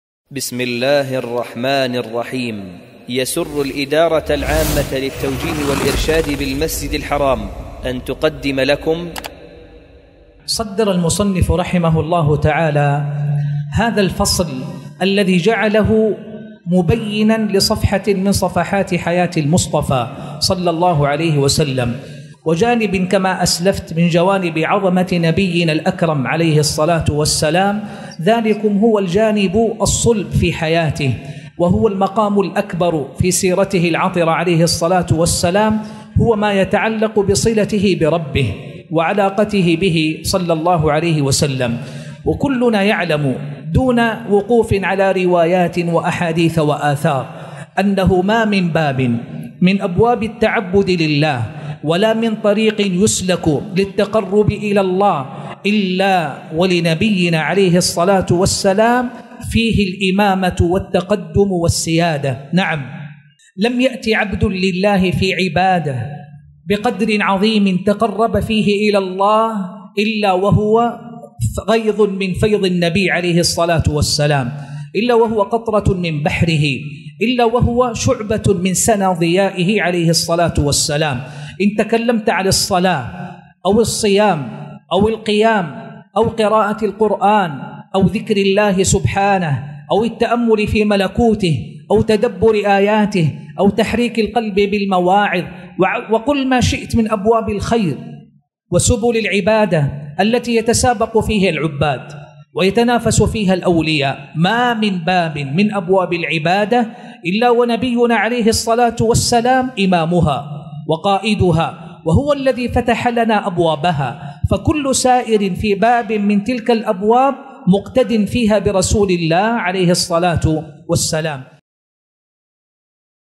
محمد صلى الله عليه وسلم قدوتنا في العبادة (كيف نكون قدوة)
ضمن برنامج كيف نكون قدوة مقطع من درس فضيلة الشيخ د. حسن بن عبدالحميد بخاري
سلسلة مقاطع تقدمها الإدارة العامة للتوجيه والإرشاد
للأئمة ومدرسي المسجد الحرام